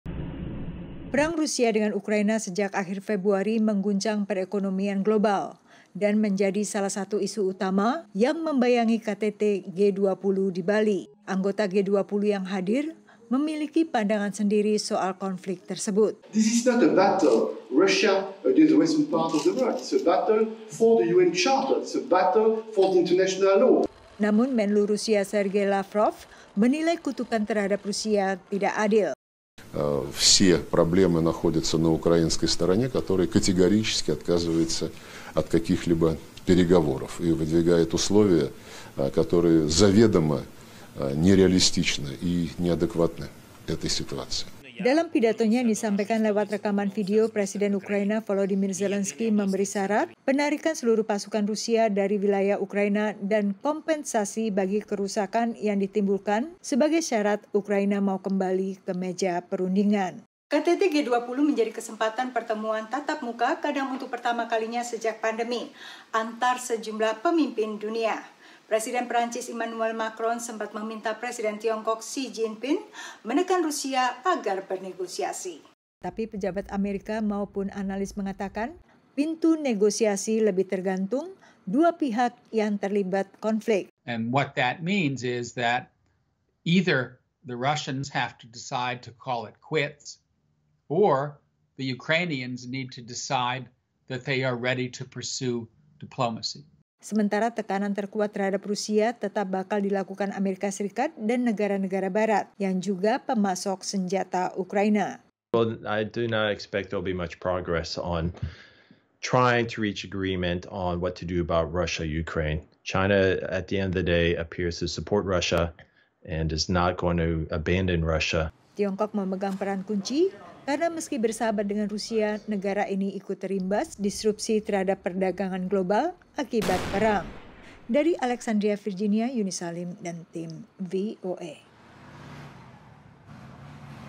Tarik Ulur Upaya G20 Mengupayakan Negosiasi Akhiri Perang Rusia-Ukraina
KTT G20 tahun ini di Bali, didominasi isu perang Rusia-Ukraina, dan lagi-lagi, anggota G20 berbeda sikap mengenai konflik ini. Apa yang bisa dilakukan negara-negara G20 untuk mendesak dimulainya negosiasi guna mengakhiri perang? Ikuti laporan tim VOA berikut ini.